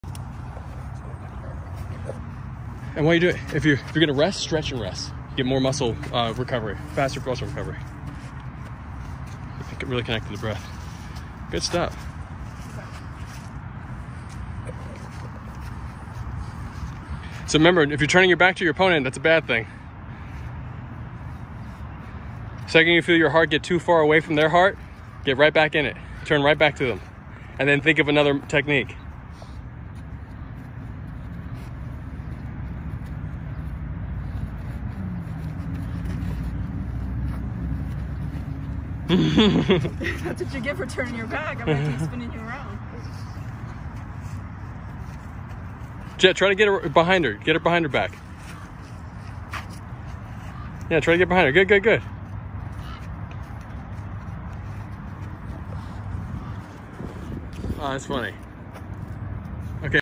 Part 4 of 6: Praying Mantis Kung Fu’s Arm Position in FREESTYLE TaiChi PushHands Grappling ☯️💪
Praying Mantis is the closest style to Wu Style Tai Chi, and many concepts are shared.